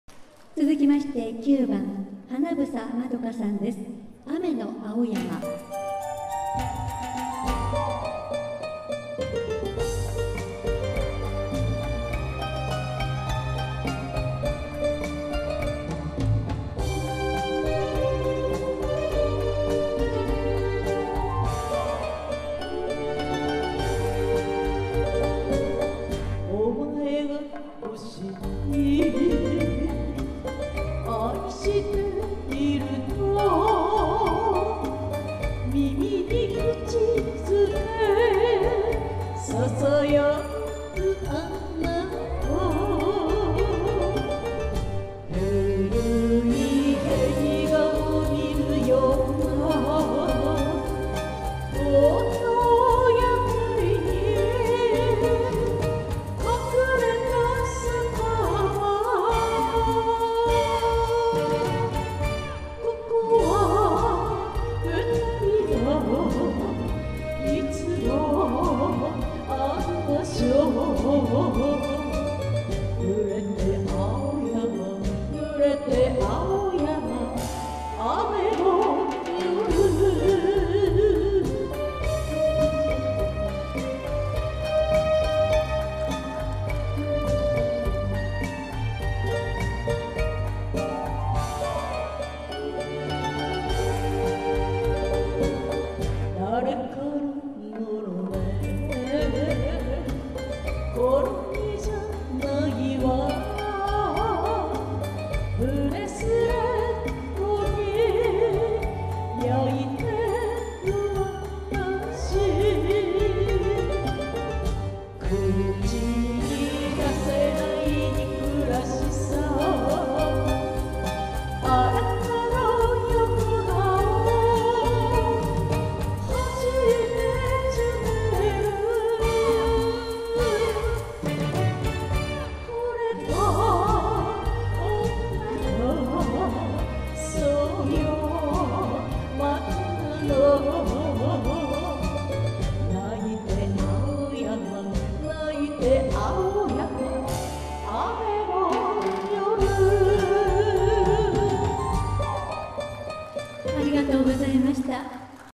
歌謡フェスタイン相模原 花房まどか
2018年5月25日相模原グリーンホールにて、歌唱